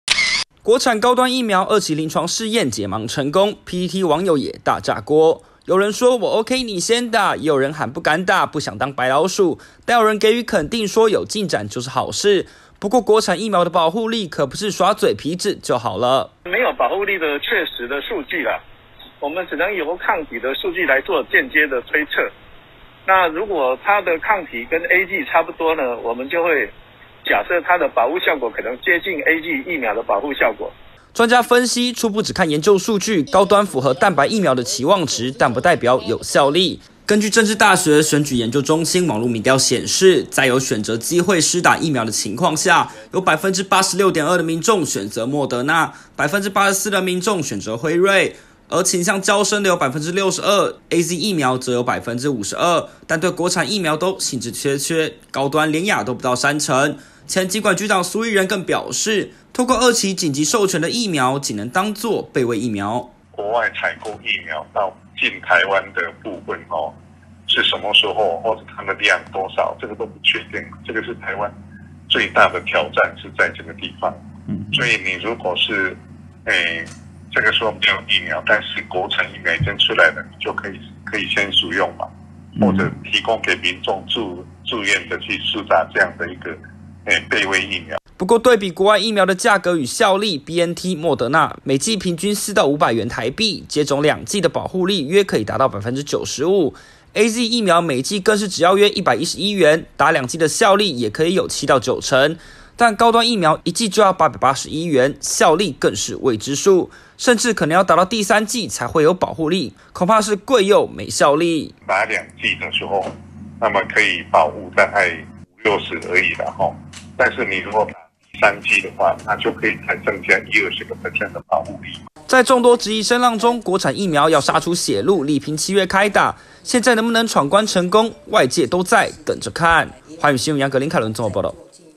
高端疫苗二期解盲成功 網友酸:我OK.你先打｜寰宇新聞20210611
現在網路社群平台PTT，針對是否敢接種高端疫苗熱烈討論，有網友大酸「官員都打國外的，叫人民打高端」、「我ok，你先打」等，顯示出國人普遍對國產疫苗信心不足，疾管局前局長蘇益仁表示，這只能當作是「備位疫苗」，不適合公費施打，現階段應該還是以國際認可疫苗為主。